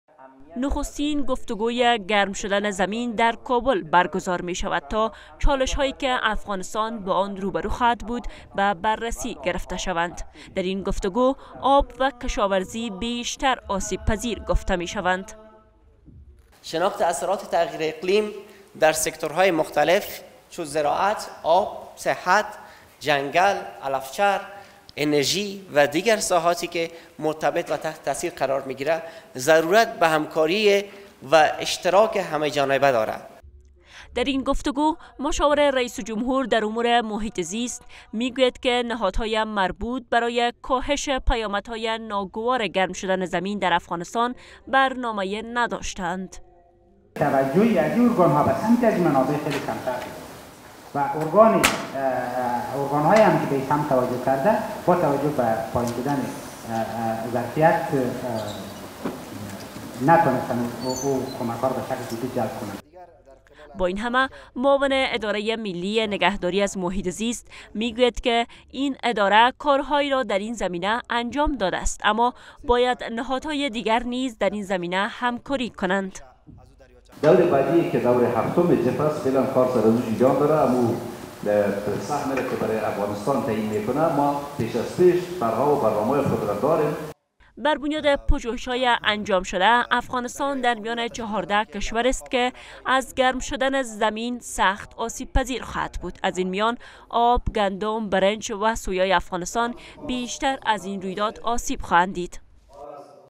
More Effort Needed To Minimize Impact Of Climate Change
آگاهان محیط زیست: افغانستان در معرض خطر تغییر اقلیم قرار دارد